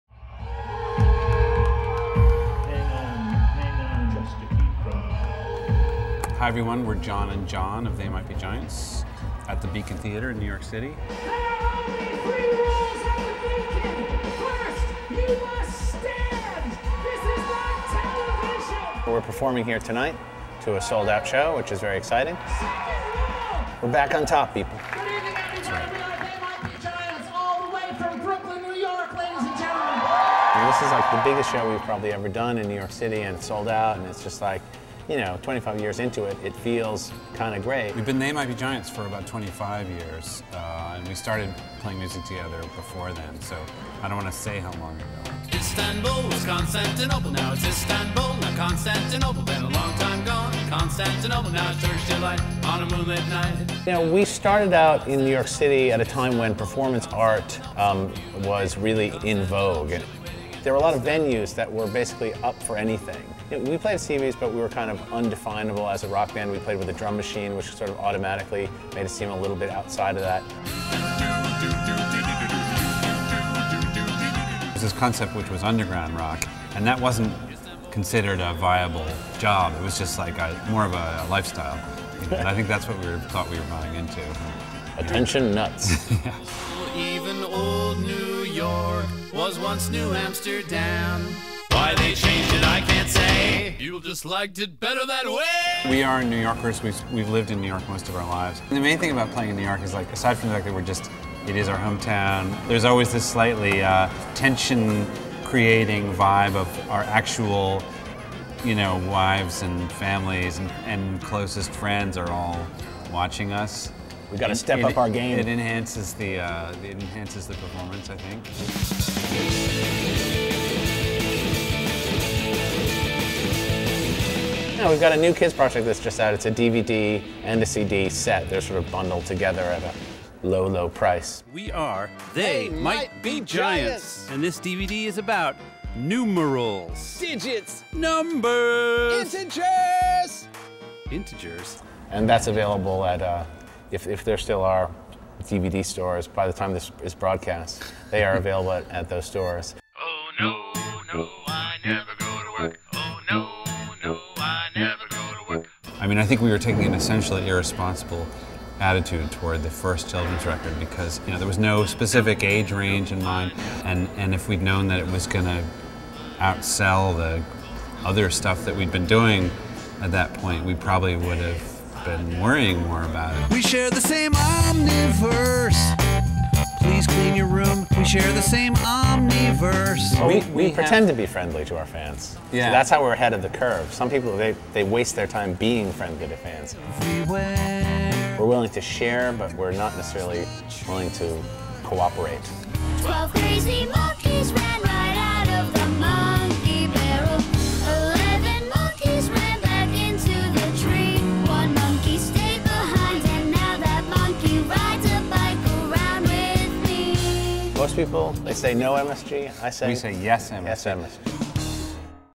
They Might Be Giants at the Beacon
They Might Be Giants take a moment to chat with MSG before their performance at the Beacon